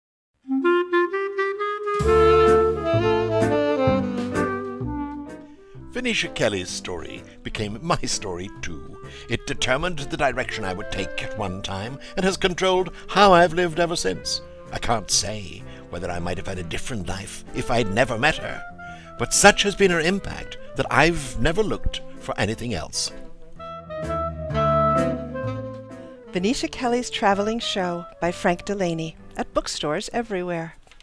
Venetia Kelly's Traveling Show by Frank Delaney
Author Frank Delaney reads from his new novel Venetia Kelly's Traveling show in this short book trailer.